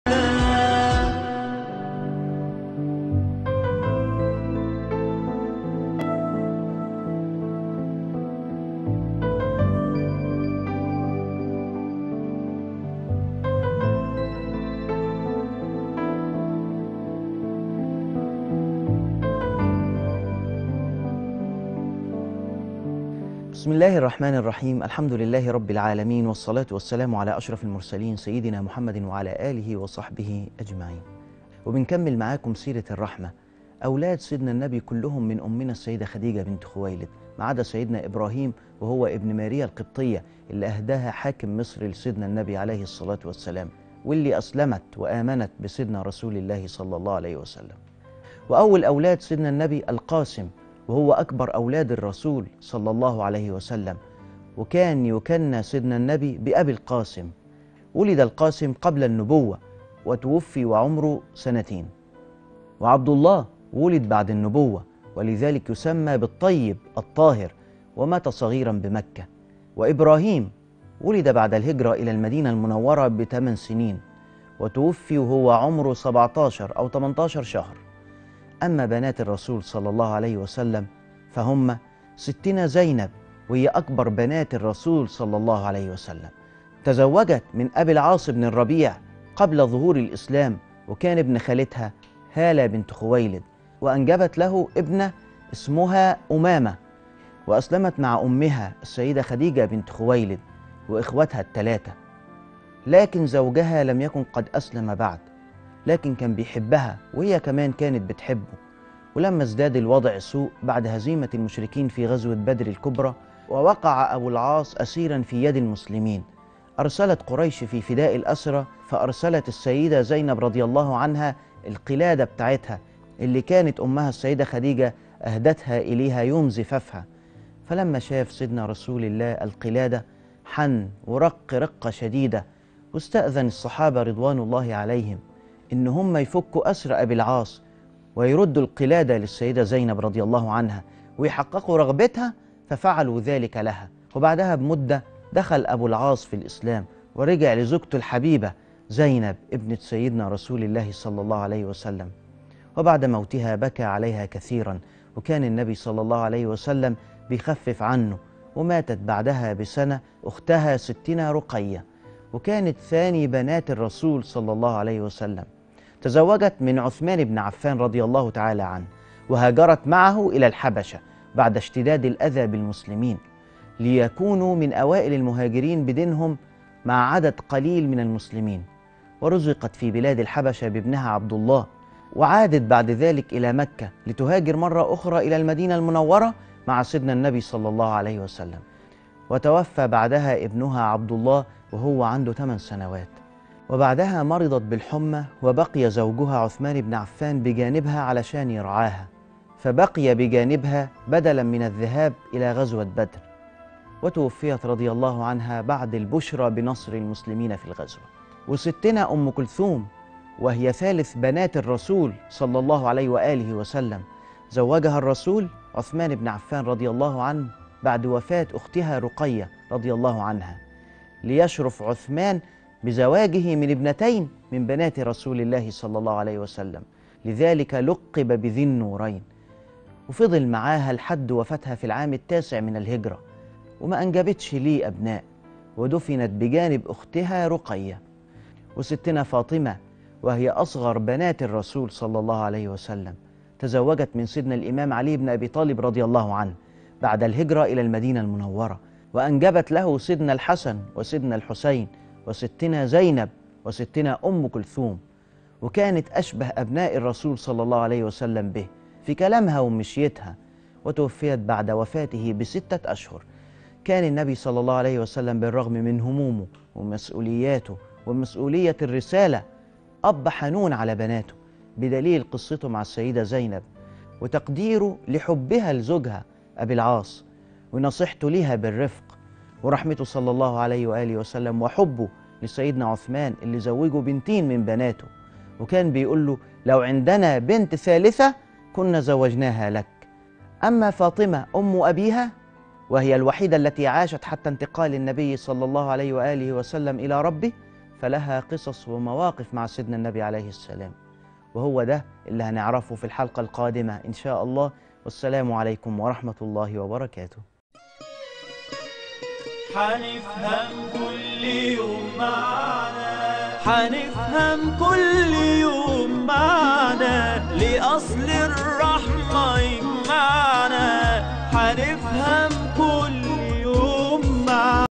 أولاد الرسول صلي الله عليه وسلم ( الحلقة الرابعة والعشرون ) | تعرف على الرحمة
الحديث عن اولاد الرسول صلي الله عليه وسلم ( الحلقه الرابعه والعشرون ) تعرف علي الرحمة بتاريخ 29/5/2019